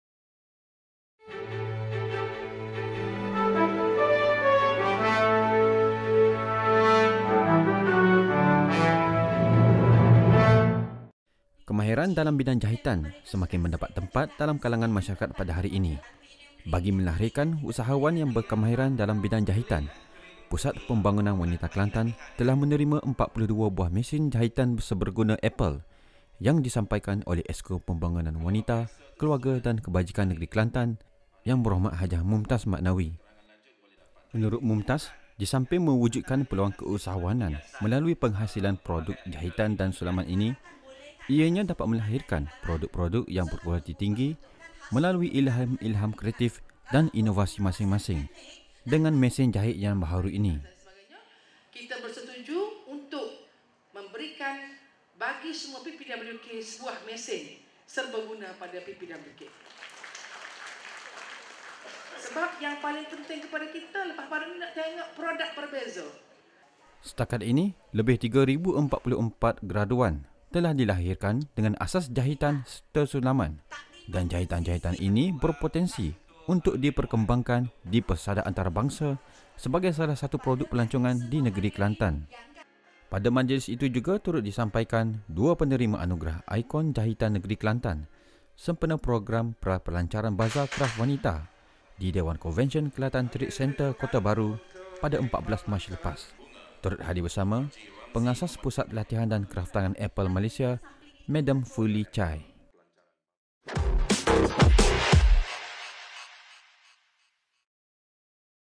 PPWK Dapat Sumbangan Mesin Jahit
Kemahiran dalam bidang jahitan semakin mendapat tempat dalam kalangan masyarakat pada hari ini.

Bagi melahirkan usahawan yang berkemahiran dalam bidang jahitan, Pusat Pembangunan Wanita Kelantan telah menerima 42 buah mesin jahitan serbaguna Epal yang disampaikan oleh Exco Pembangunan Wanita, Keluarga dan Kebajikan Negeri Kelantan.

Menurut Mumtaz, disamping mewujudkan peluang keusahawanan melalui penghasilan produk jahitan dan sulaman ini, ianya dapat melahirkan produk-produk yang berkualiti tinggi melalui ilham-ilham kreatif dan inovasi masing-masing dengan mesin jahit yang baharu ini.

Setakat ini, lebih tiga ribu empat puluh empat (3044) graduan telah dilahirkan dengan asas jahitan serta sulaman dan jahitan-jahitan ini berpotensi untuk diperkembangkan di persada antarabangsa sebagai salah satu produk pelancongan di negeri Kelantan.

Pada majlis itu juga turut disampaikan dua penerima anugerah ikon jahitan negeri Kelantan sempena  Program Pra-Pelancaran Bazar Kraf Wanita di Dewan Konvensyen Kelantan Trade Centre, Kota Bharu pada 14 Mac lepas.

Turut hadir sama, Pengasas Pusat Latihan dan Kraftangan Epal Malaysia, Madam Fully Chye.